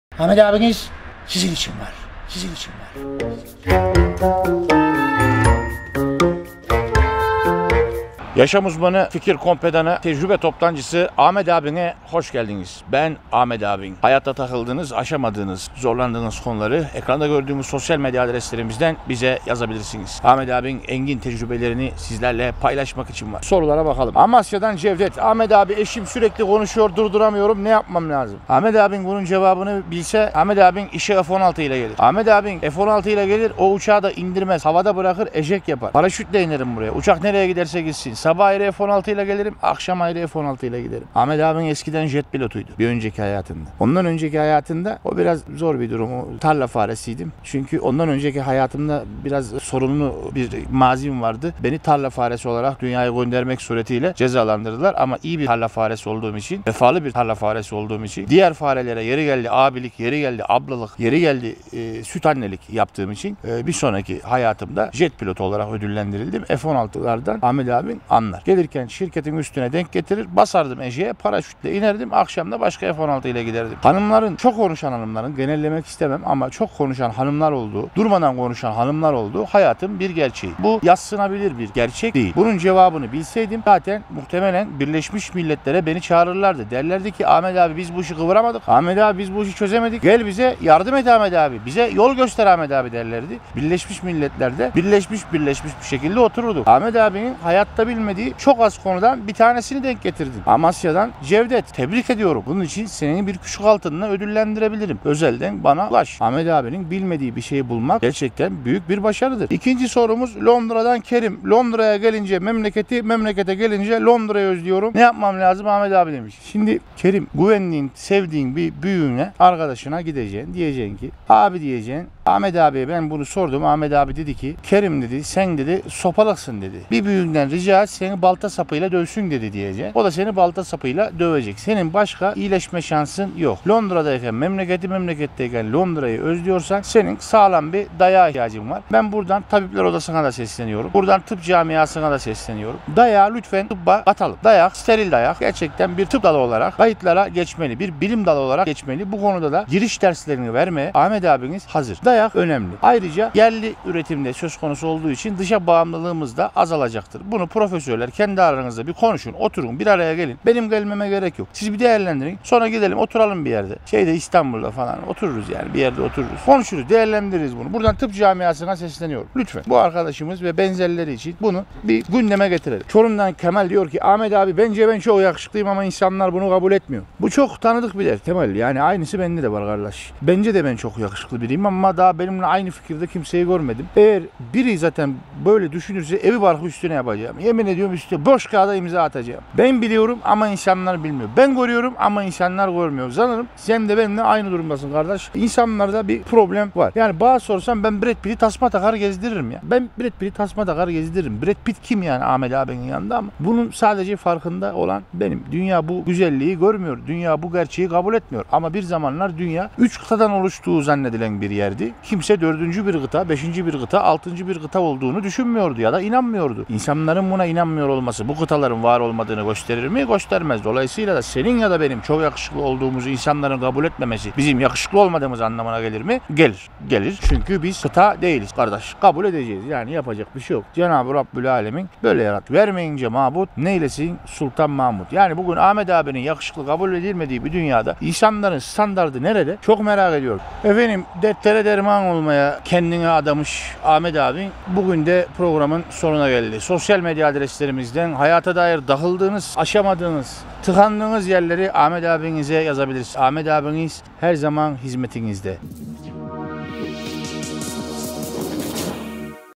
Yaşam Eksperi&Fikir Kompetanı Ahmet Ağbin-6. Bölüm: Memleket özlemi
Yaşanabilecek her şeyi yaşamış adam; Ahmet Ağbin, tecrübelerini aktarıyor. Ahmet Ağbin, önceki hayatında neydi? Memleket özlemi nasıl giderilir? Hayatın Kapalıçarşısı Ahmet Ağbin, soruları yanıtlıyor. İşte 6. bölüm...